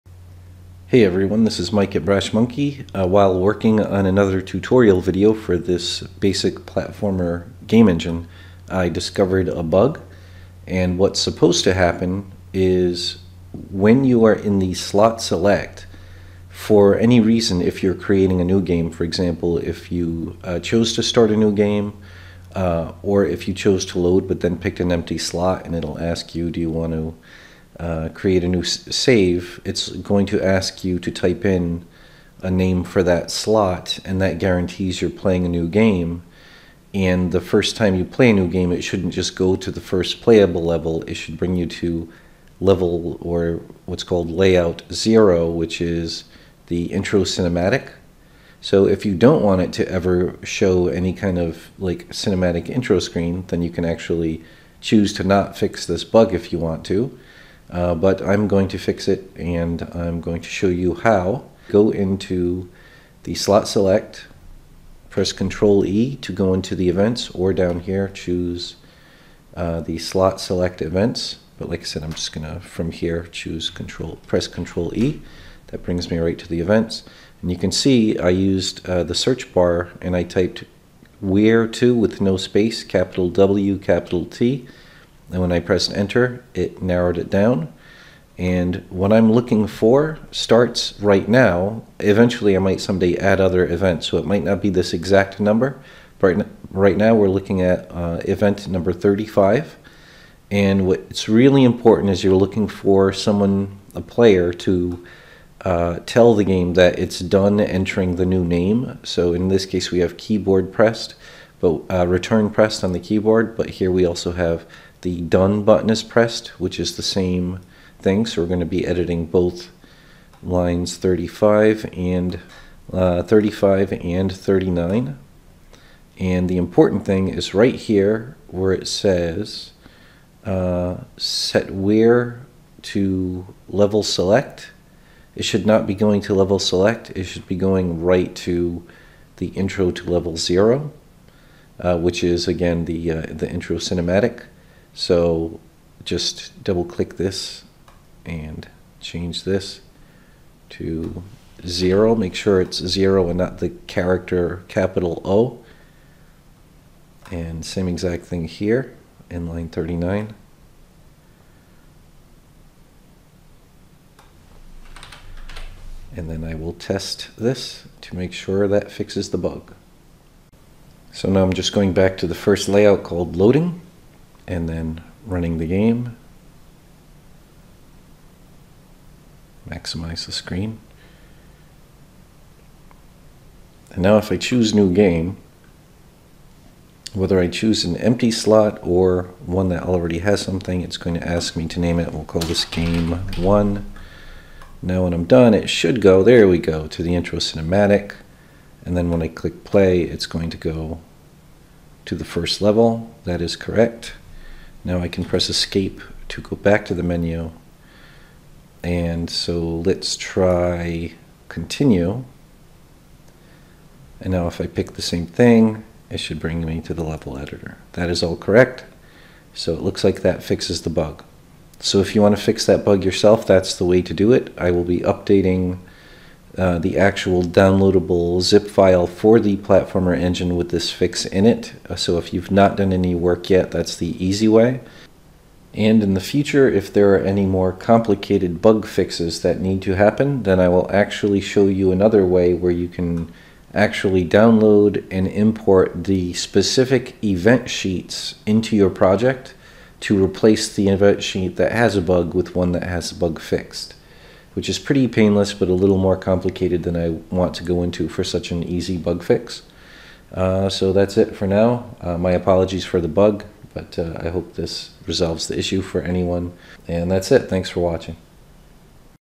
Quick bug fix for the Basic Platformer Game Engine for construct 2 or 3
My apologies for my slow pace etc, I was recording this at 4 am and had not slept yet. ;)
-Mike